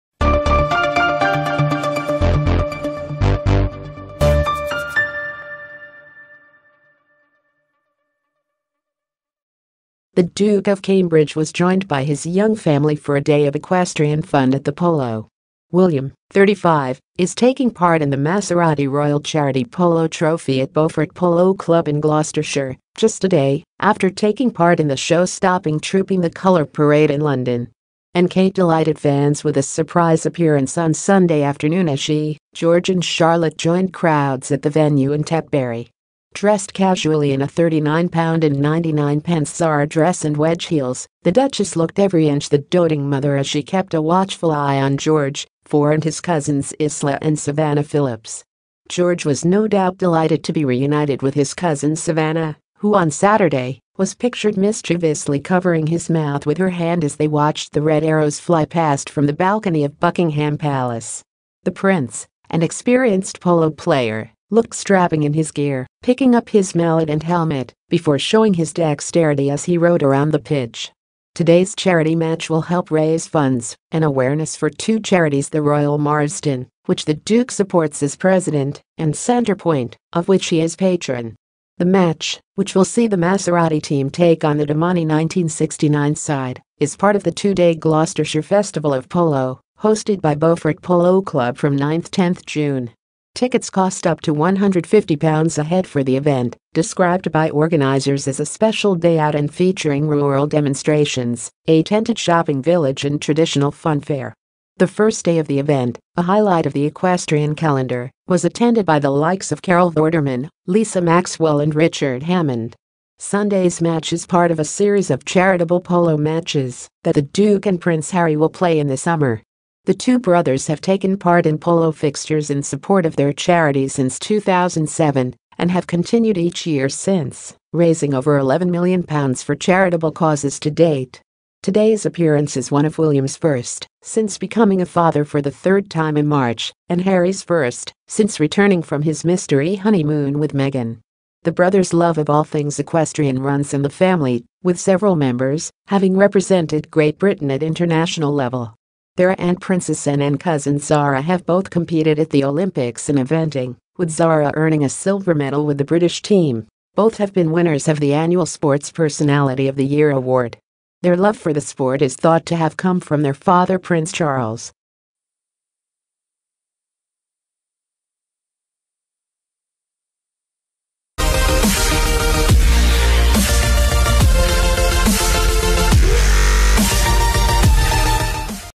Kate Middleton is the image of a doting mother as she watches polo with playful George and Charlotte
William, 35, was competing in the Maserati Royal Charity Polo Trophy at Beaufort Polo Club in Gloucestershire, just a day after taking part in the show-stopping Trooping the Colour parade in London.